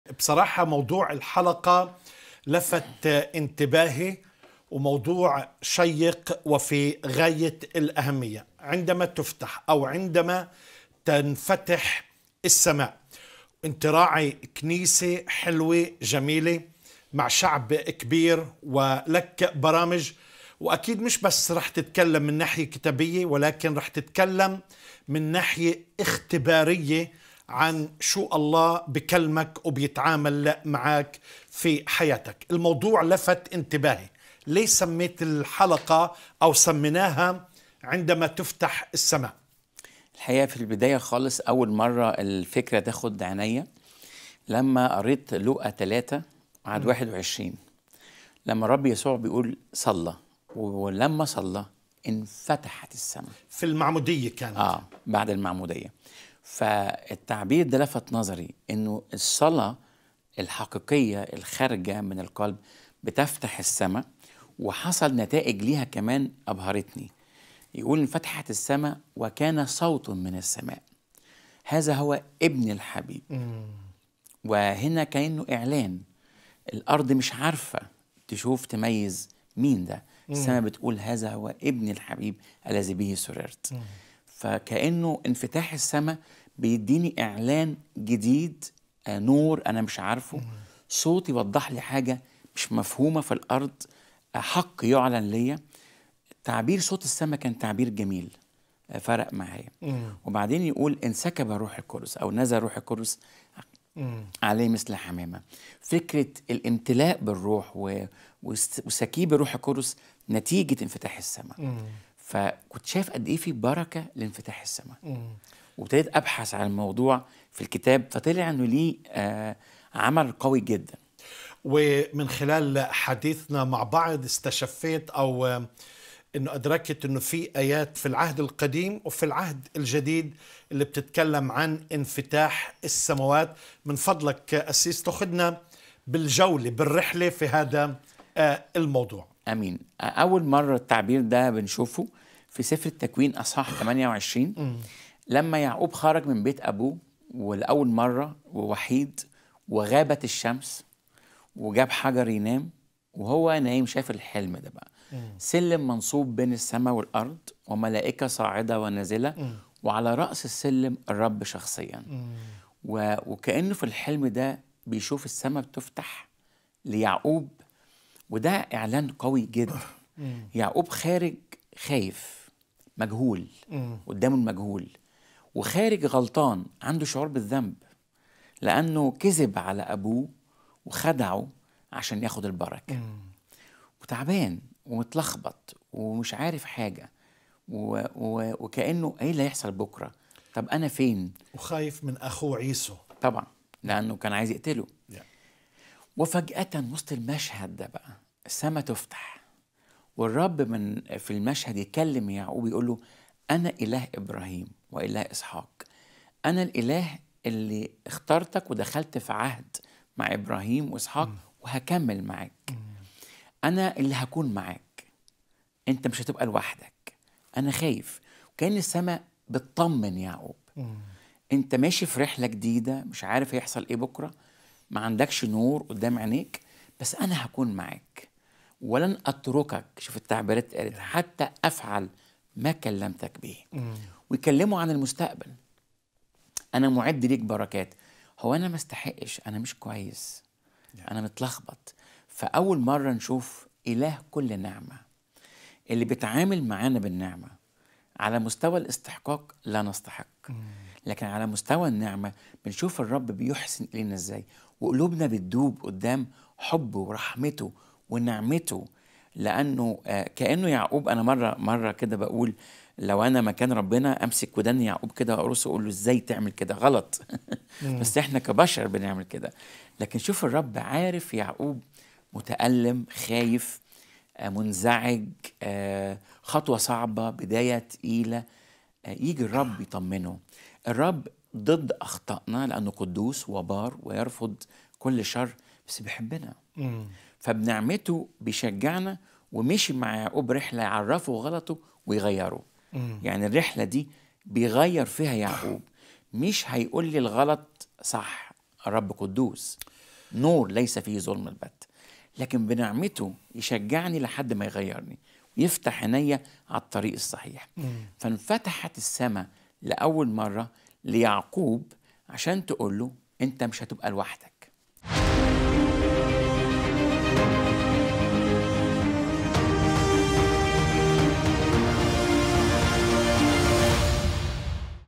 01 ما هي المرة الأولى التي ورد فيها تعبير انفتحت السماء في الكتاب المقدس؟
ما هي المرة الأولى التي ورد فيها تعبير انفتحت السماء في الكتاب المقدس؟

📌يمكنك التواصل معنا بصورة شخصية عن طريق رسائل الواتساب👇
WhatsApp الواتساب:    🆆🅷🅰🆃🆂🅰🅿🅿